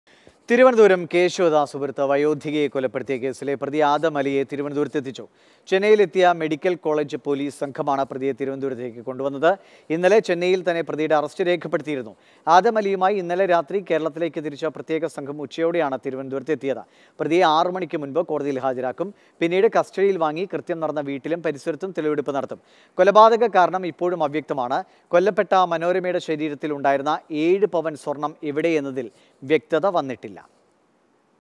കേശവദാസപുരത്ത് വയോധികയെ കൊലപ്പെടുത്തിയ പ്രതിയെ തിരുവനന്തപുരത്ത് എത്തിച്ചു | Mathrubhumi News
തിരുവനന്തപുരം കേശവദാസപുരത്ത് വയോധികയെ കൊലപ്പെടുത്തിയ കേസിലെ പ്രതി ആദം അലിയെ തിരുവനന്തപുരത്ത് എത്തിച്ചു. ചെന്നൈയിലെത്തിയ മെഡിക്കൽ കോളേജ് പോലീസ് സംഘമാണ് പ്രതിയെ തിരുവനന്തപുരത്തെത്തിച്ചത്.

Happy viewing!